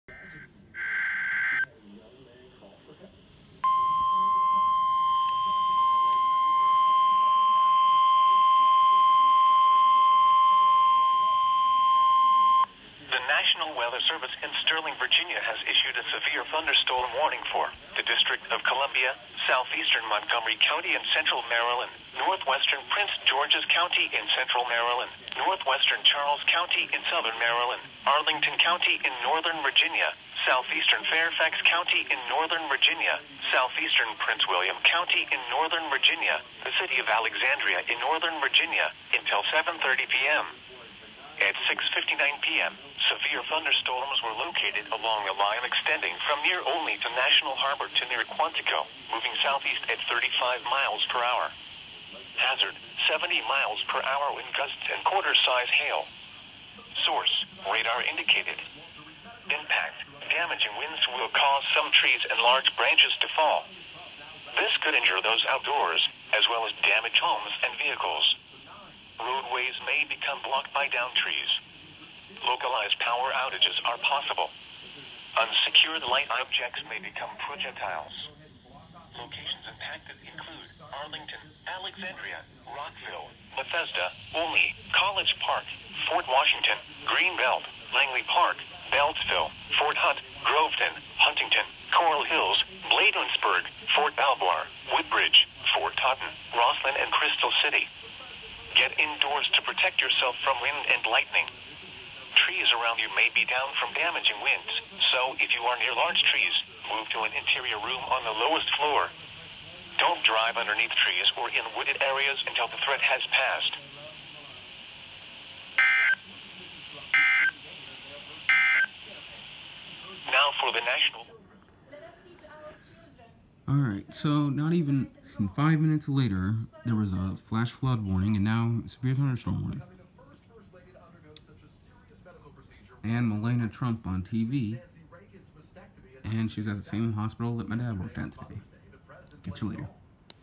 Another activation
Not even 5 minuets later my radio went off again. I need an actual weather radio. I’m saving for a reecom r1650, an r500, and a sangean cl100